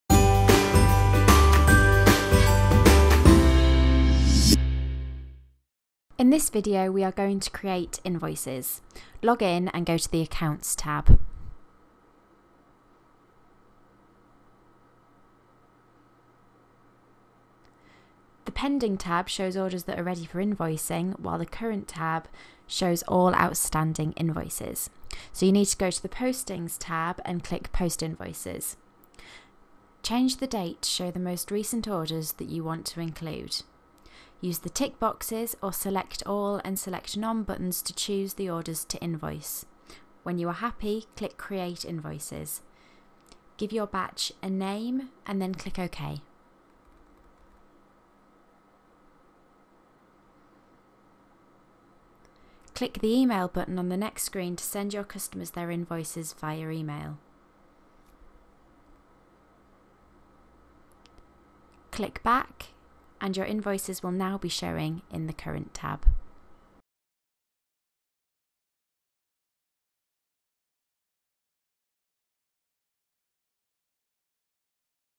Strelitzia Tutorials: Creating Invoices
Strelitzia Tutorial:

In this step-by-step tutorial we will show you how to create an invoice for all of your credit customers in just a few clicks within Strelitzia.